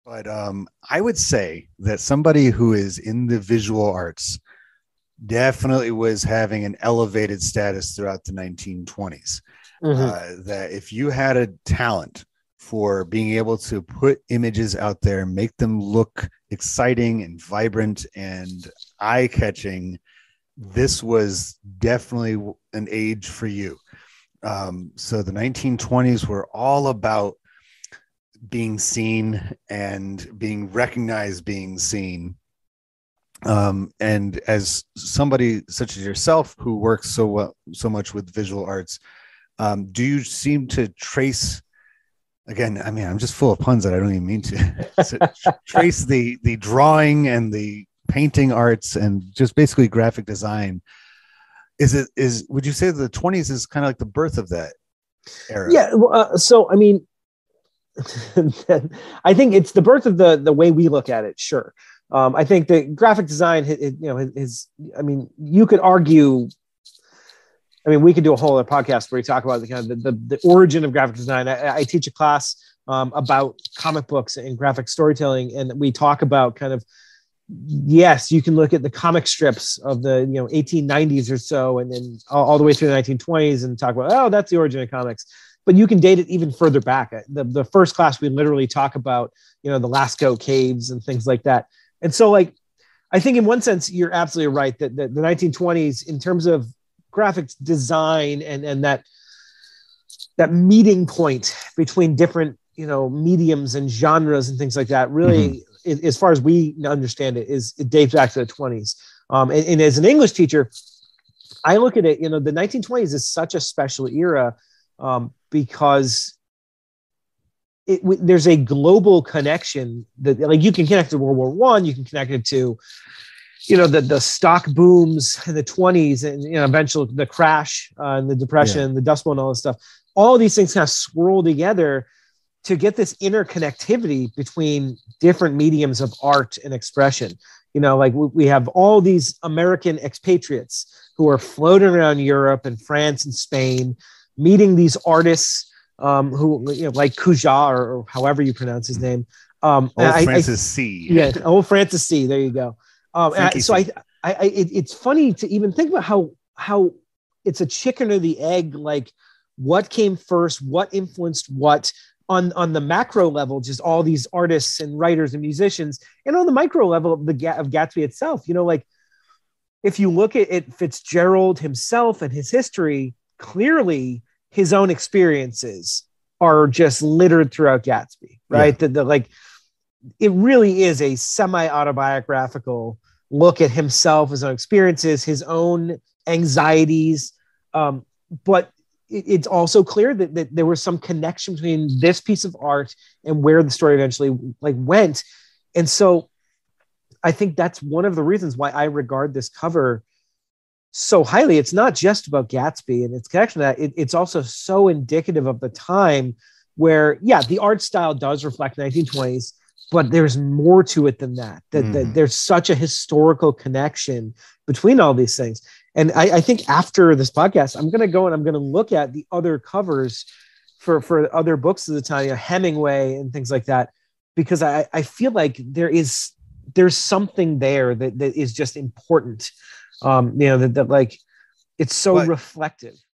The EPS Podcast S1E13 "The Legend of Zelda" Clip
Episode 13: "The Legend of Zelda" Analyzing Francis Cugat's "Celestial Eyes" as a Primary Source.

 I'm joined again by Andrew to look at how the cover art for 1925's The Great Gatsby is just as useful as a primary source of 1920s America as the book it graces.  Created fairly early in the history of graphic design, Cugat's painting was deeply intertwined with the story The Great Gatsby itself, and may have informed some of its most iconic components.

Listen to the full episode wherever you get your podcasts